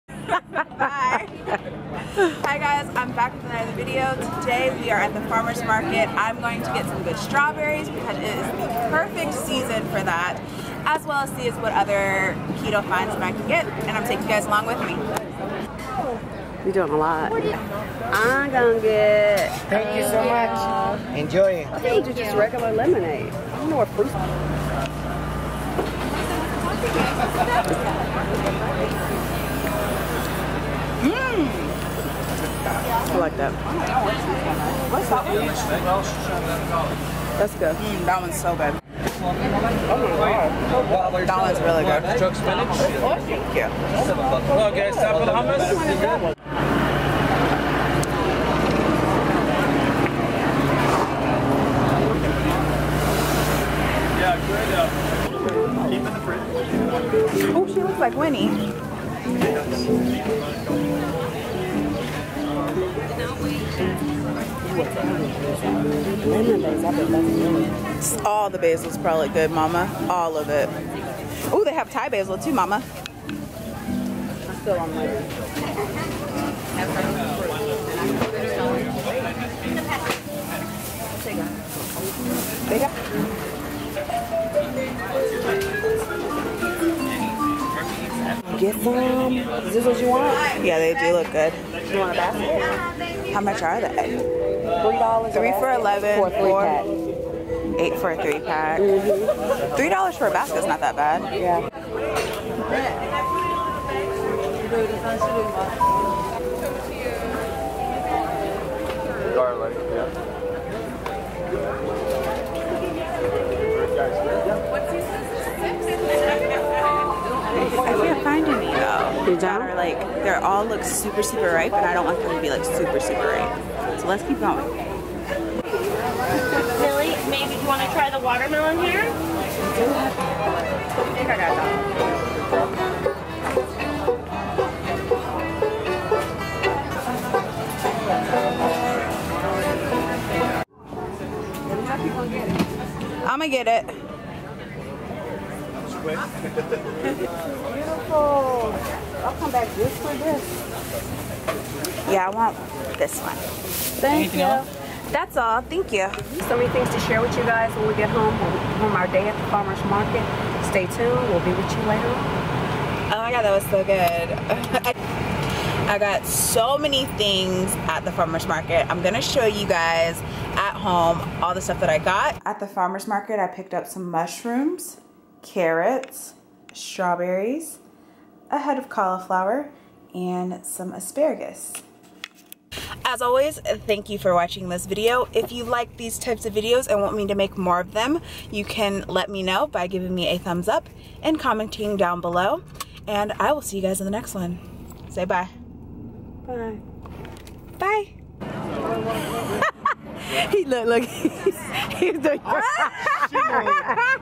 Farmer's Market Shopping | Low Carb Shopping | Keto Grocery Haul
Thumbs up if you want me to take you on more grocery runs!

Summer is the best time to shop local and farmers markets make it so easy to get affordable fruits and veggies. All of these cost under $20!

SNAPCHAT ‣ keilakandid